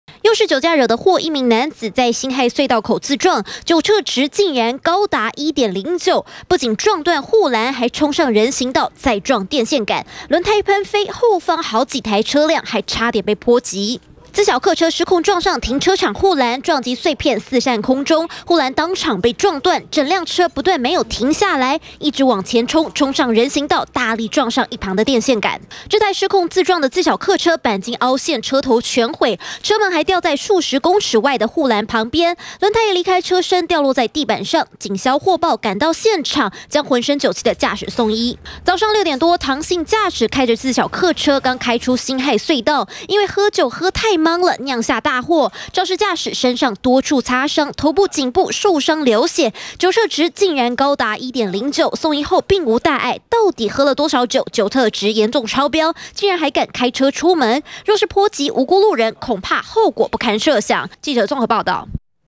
20191028中天新聞 辛亥隧道自撞車禍 駕駛酒測值高達1.09
上午六點多，台北辛亥隧道發生一起自撞車禍，一名五十多歲的男姓駕駛，剛開出隧道口，就失控撞上停車場護欄，又衝上人行道撞上電線桿，驚險撞擊瞬間，讓後方車輛駕駛嚇壞，而這名男子送醫抽血檢驗後，酒側值竟高達1.09。